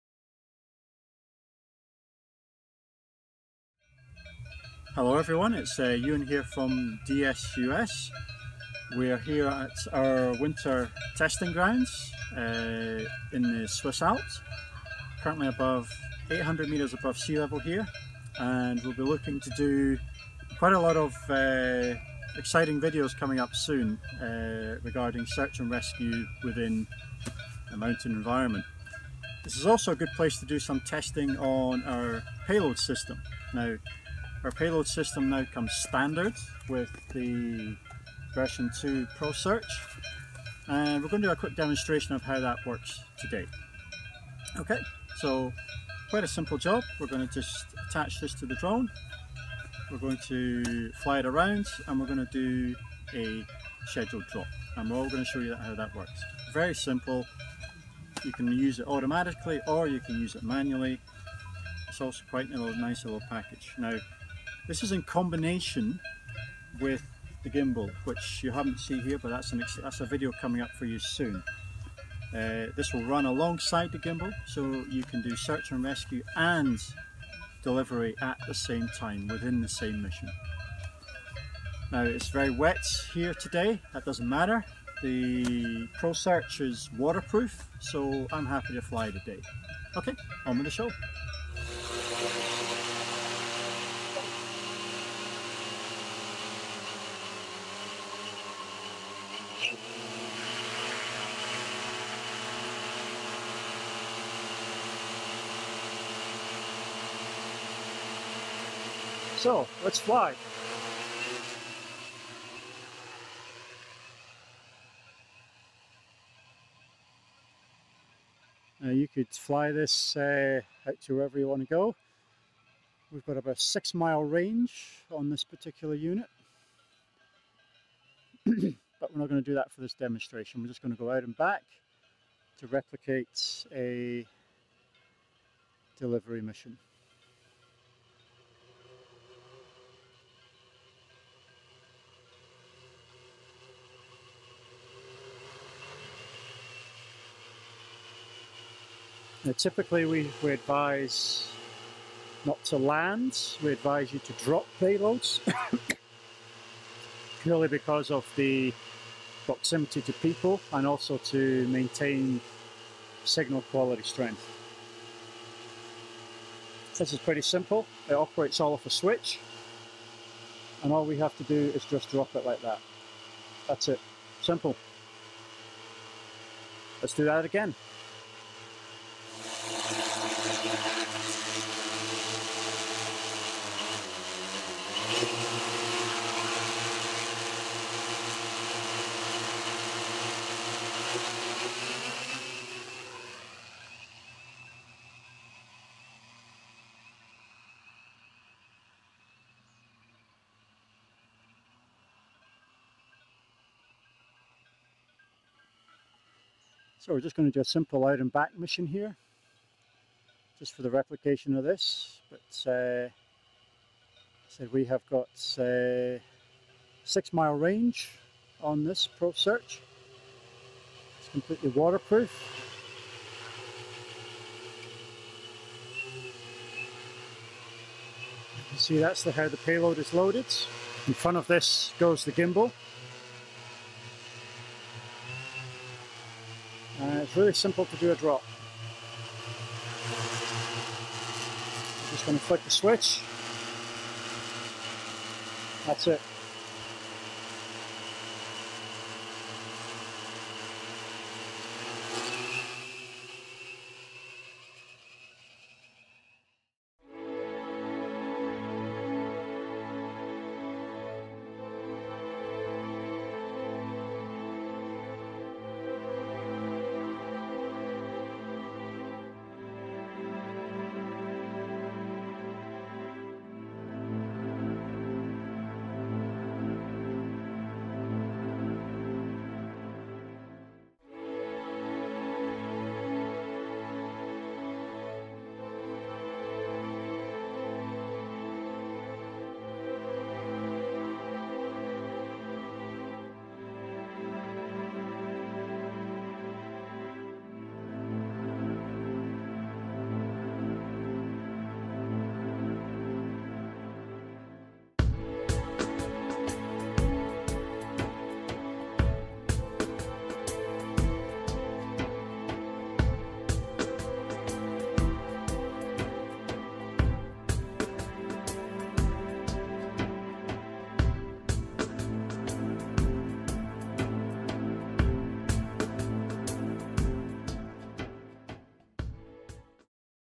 ProSearch XLF + Prodropper
A quick demonstration of our new ProSearch platform, using the now-standard prodropper payload system. This airframe allows SAR operators to perform both active searching and emergency pack deployment in the same mission.